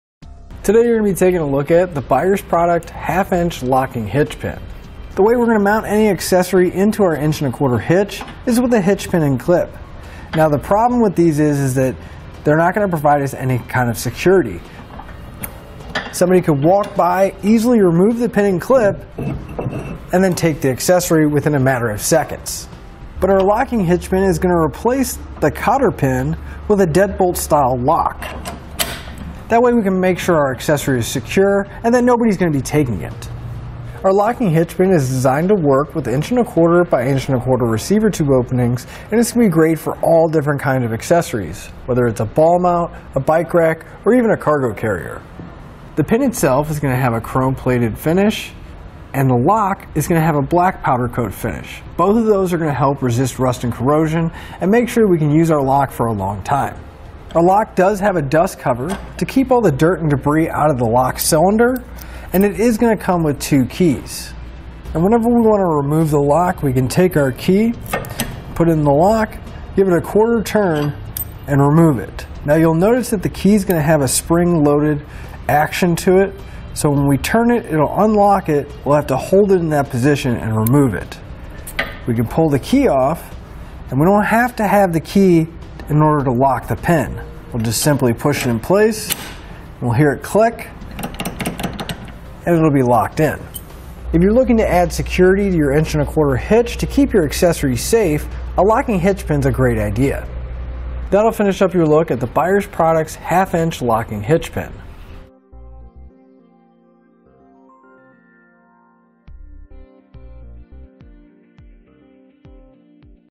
etrailer | Buyers Products 1/2" Locking Hitch Pin Review
Click for more info and reviews of this Buyers Products Trailer Hitch Lock:

Today, you're going to be taking a look at the Buyer's product 1/2 inch locking hitch pin. The way we're going to mount any accessory into our inch and 1/4 hitch, is with a hitch pin and clip. Now the problem with these is, is that they're not going to provide us any kind of security. Somebody could walk by, easily remove the pin and clip and then take the accessory within a matter of seconds. But, our locking hitch pin is going to replace the cotter pin with a deadbolt style lock, that way, we can make sure our accessory is secure and that nobody's going to be taking it.A locking hitch pin is designed to work with inch and 1/4 by inch and 1/4 receiver to openings and this will be great for all different kind of accessories, whether it's a ball mount, a bike rack or even a cargo carrier. The pin itself, is going to have a chrome plated finish and the lock is going to have a black powder coat finish.

Both of those are going to help resist rust and corrosion and make sure we can use our lock for a long time.The lock does have a dust cover, to keep all the dirt and debris out of the lock cylinder and it is going to come with two keys. And, whenever we want to remove the lock, we can take our key, put it in the lock, give it a quarter turn and remove it. Now, you'll notice that the key's going to have a spring loaded action to it, so when we turn it, it'll unlock it, we'll have to hold it in that position and remove it. We can pull the key off and we don't have to have the key, in order to lock the pin. We'll just simply push it in place and we'll hear it click and it'll be locked in.If you're looking to add security to your inch and 1/4 hitch, to keep your accessories safe, a locking hitch pin's a great idea.

That'll finish up your look at the Buyer's products 1/2 inch locking hitch pin.